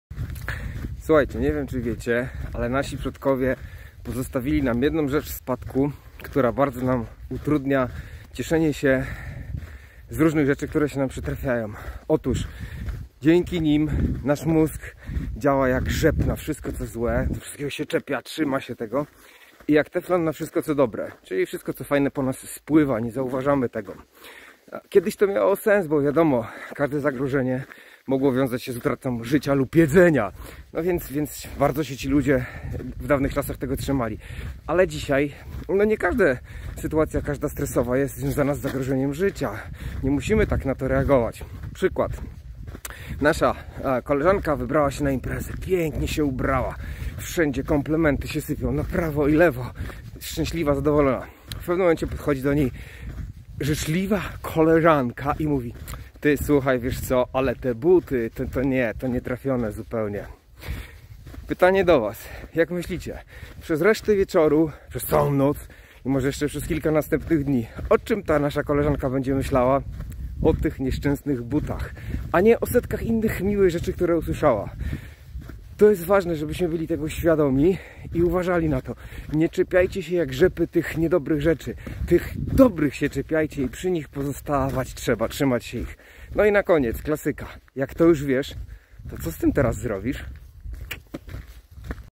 Co masz w głowie - rzep czy teflon?
Nasi przodkowie zostawili nam coś w spadku.
Niestety to "coś" często nam w obecnych czasach nie służy 🙁